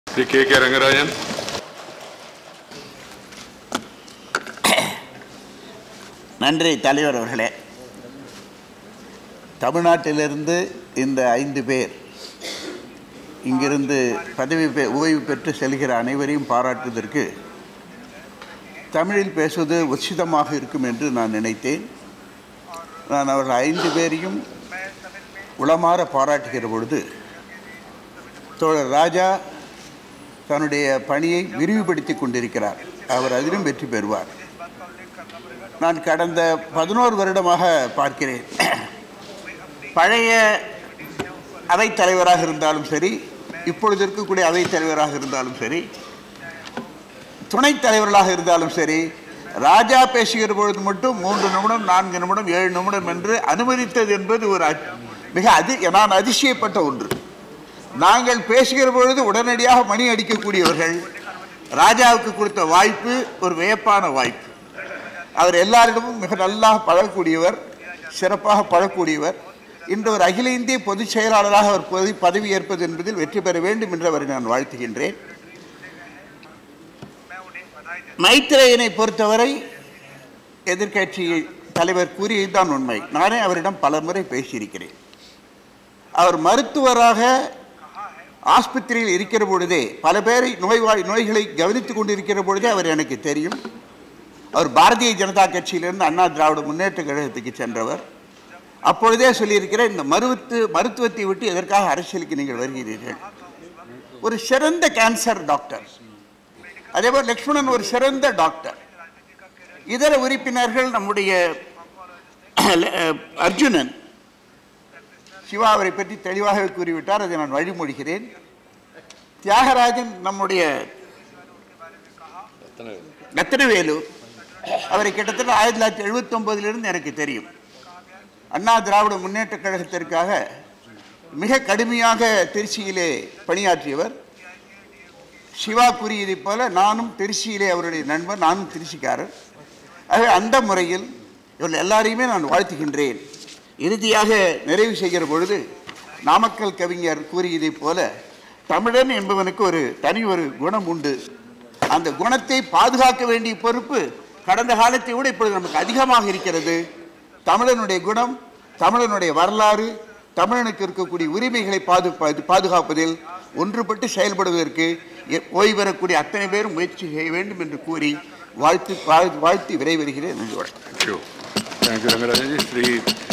T K Rangarajan bids farewell to retiring Rajya Sabha members
Description: On July 24, 2019, the Upper House of Parliament bid farewell to its retiring members.